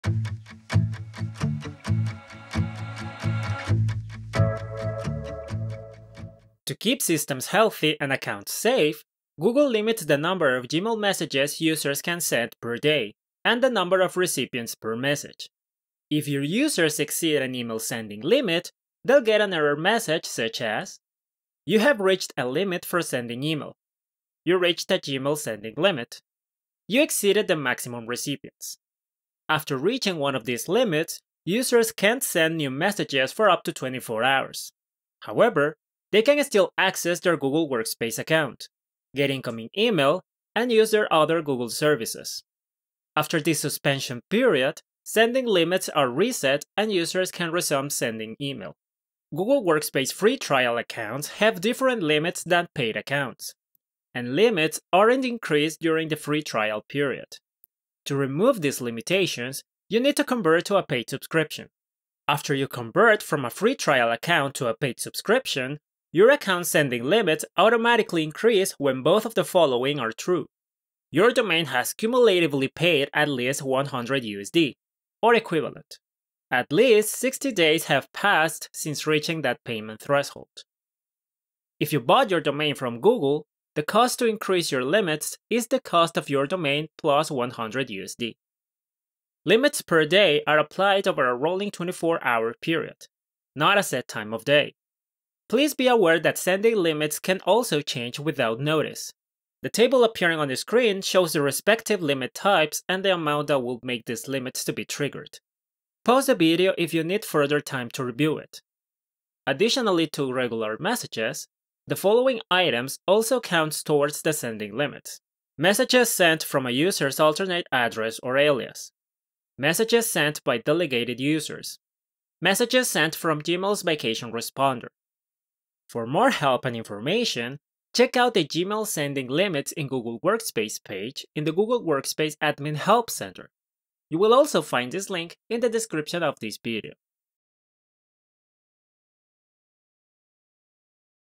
Gmail sending limits in Google Workspace
Having issues while sending emails through Gmail while having Google Workspace? You may be affected by a Gmail sending limit . In this video we’ll tell you more information about how Gmail sending limits works on Google Workspace. You can learn more about this in the following Help Center article